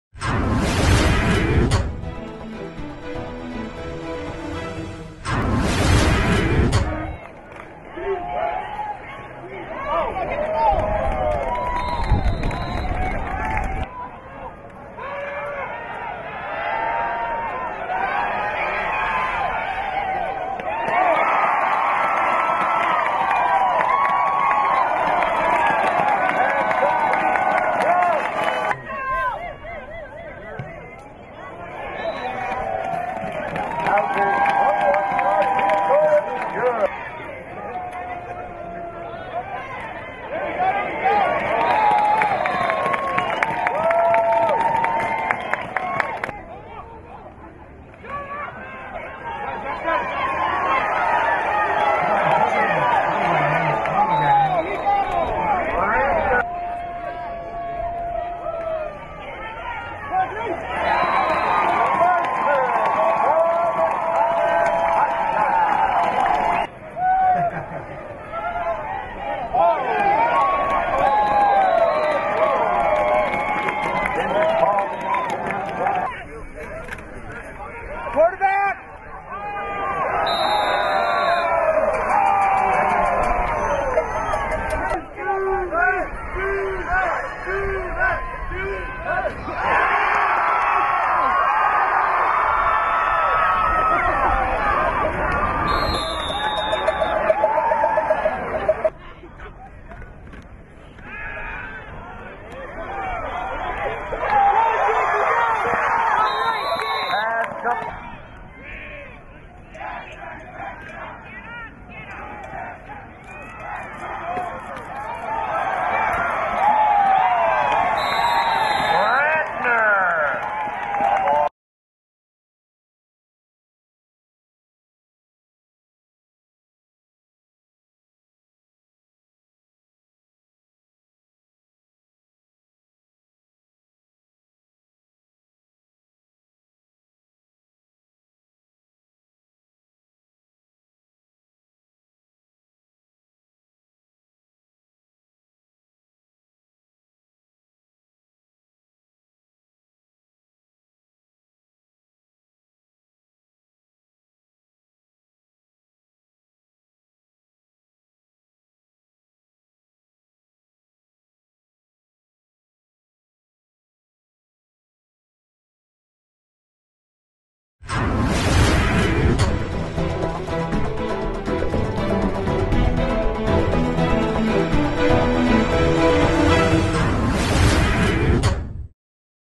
Cobber Football Highlights vs. Gustavus - Nov. 14, 2009
Highlights of Concordia's season finale vs. Gustavus on Nov. 14, 2009